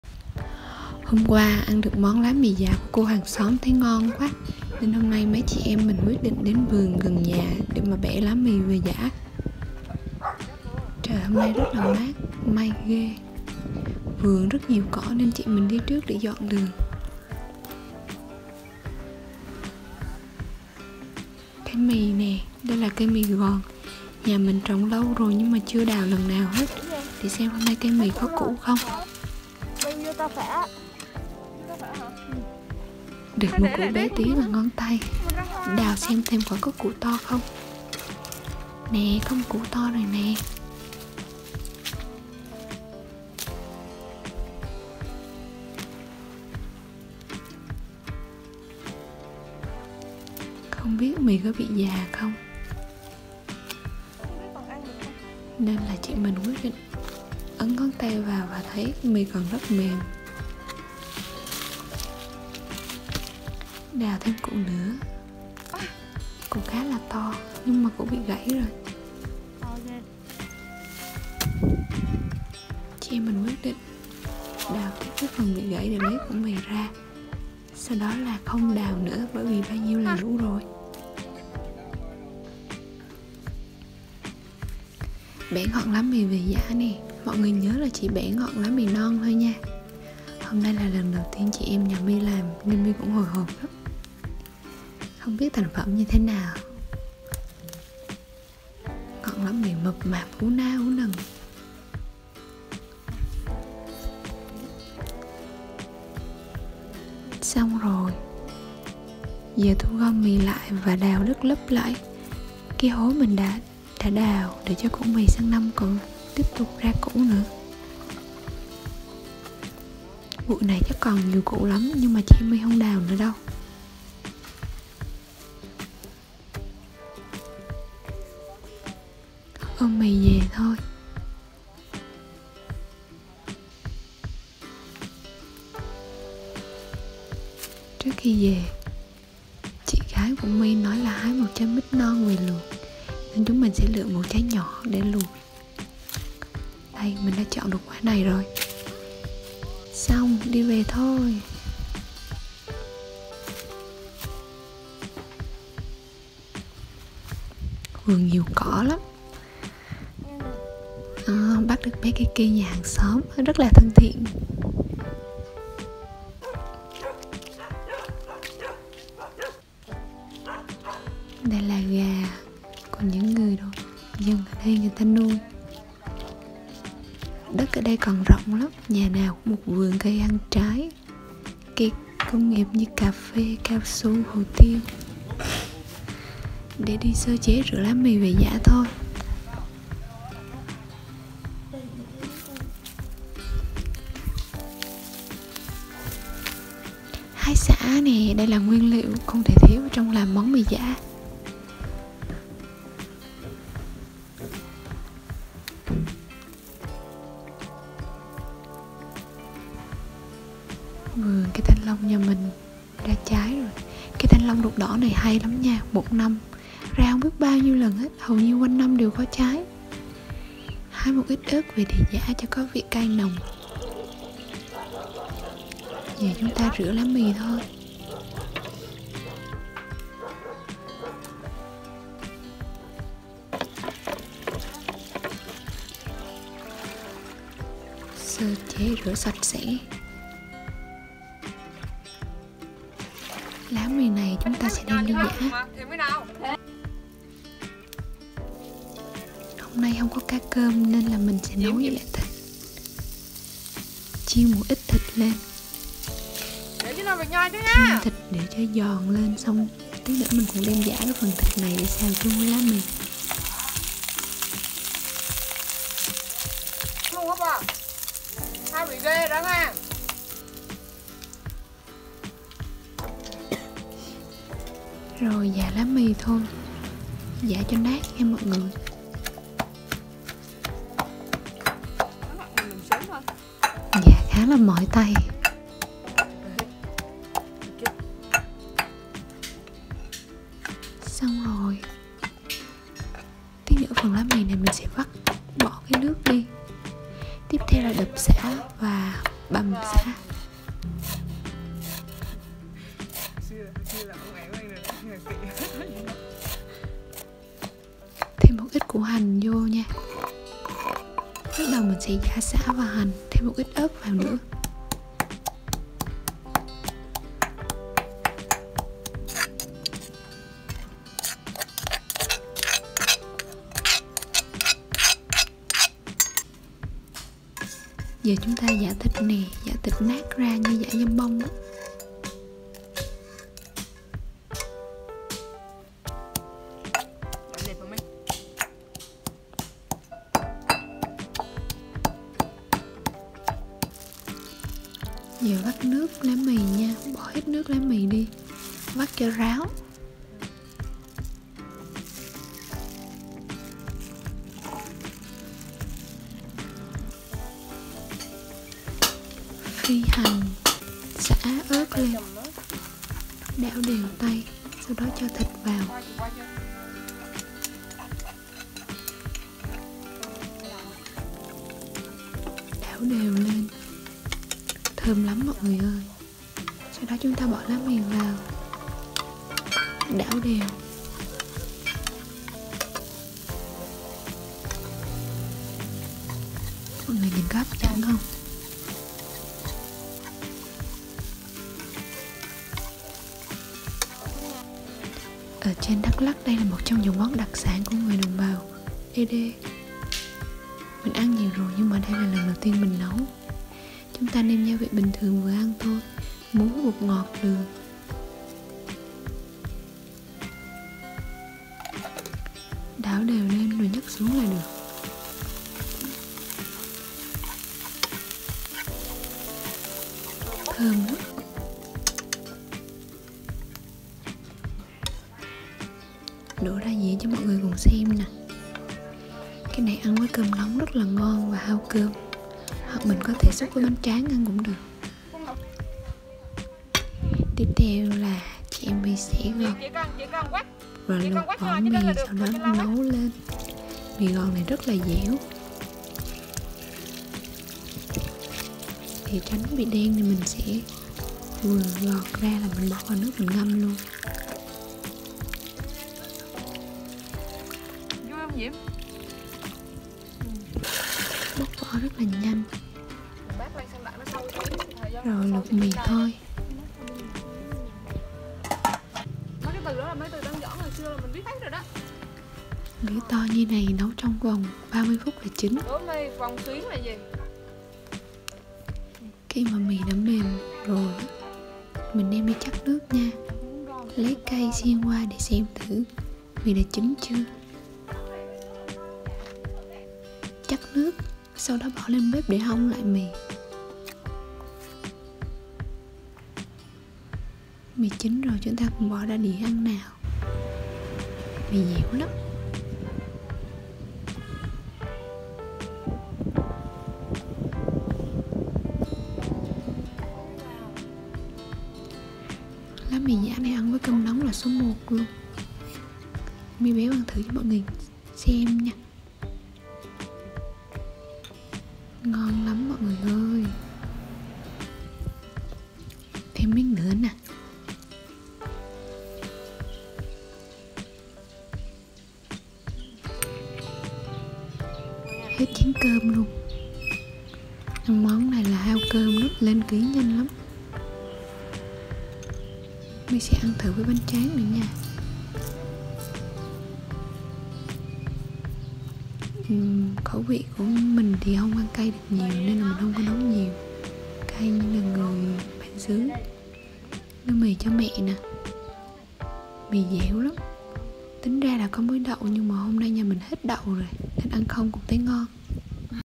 LÁ MÌ (SẮN) BẠN ĐÃ ĂN BAO GIỜ CHƯA? MÓN NGON ĐỘC ĐÁO TỪ NGƯỜI Ê ĐÊ QUÊ HƯƠNG ĐAK LAK
LÁ MÌ GIÃ PHONG CÁCH ĐAK LAK ĂN VÀO LÀ MÊ ĐIẾU ĐỔ!